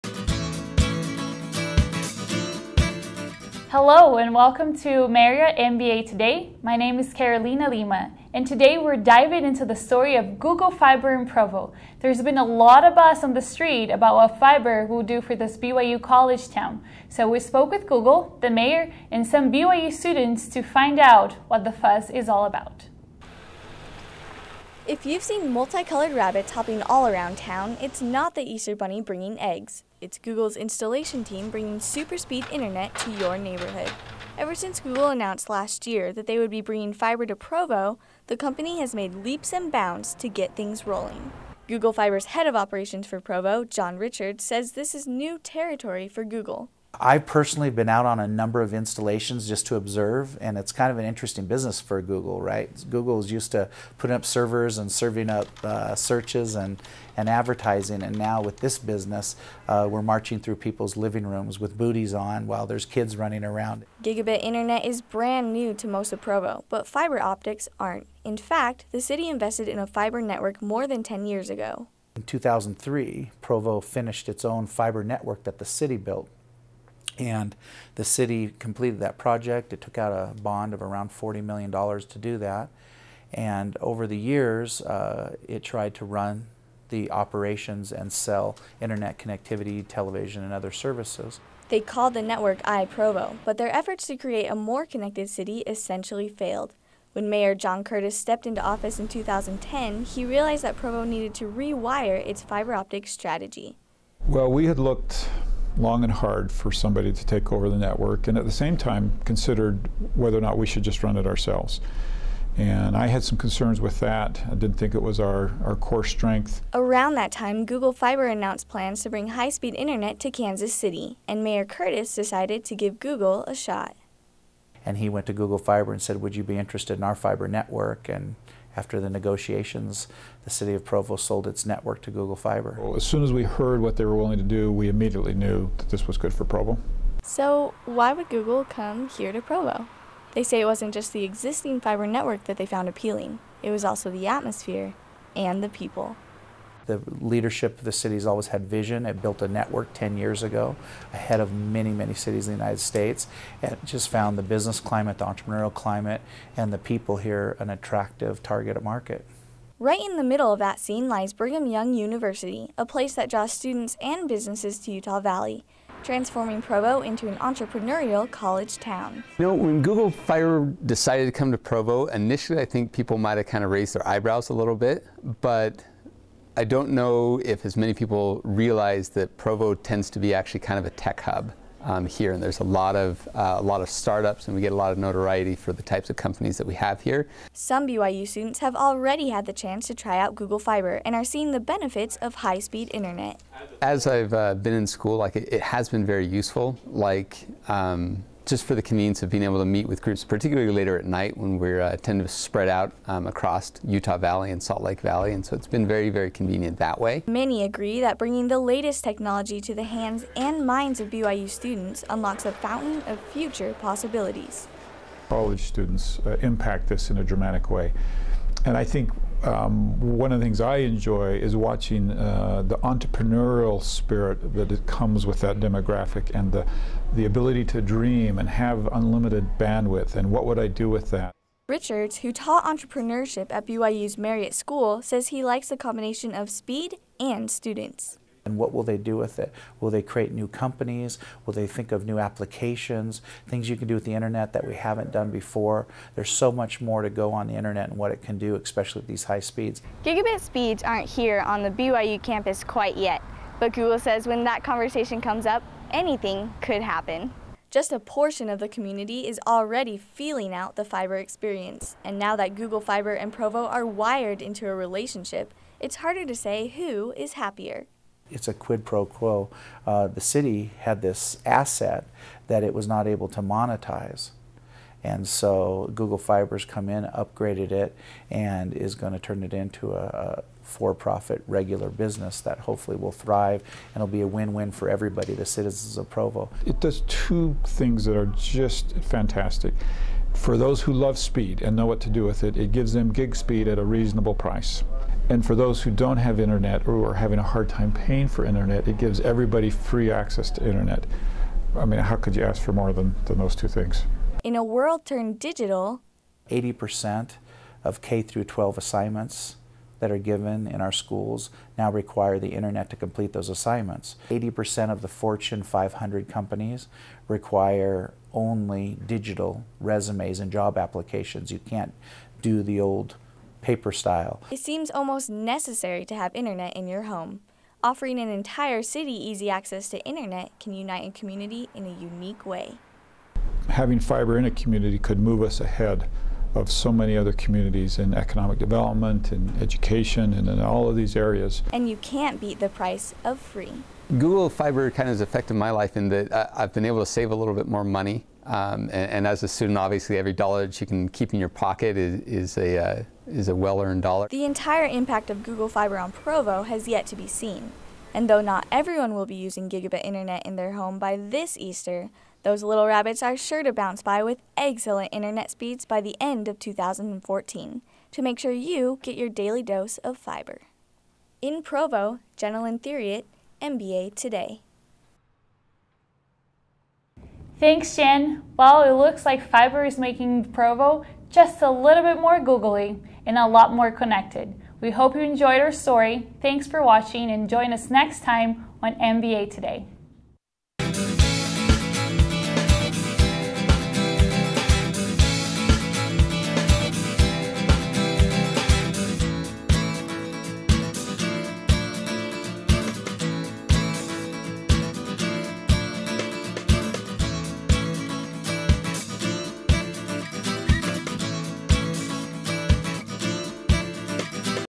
Google Fiber comes to Provo
Goggle Fiber makes its debut -- helping students, job seekers, and companies in Provo, Utah.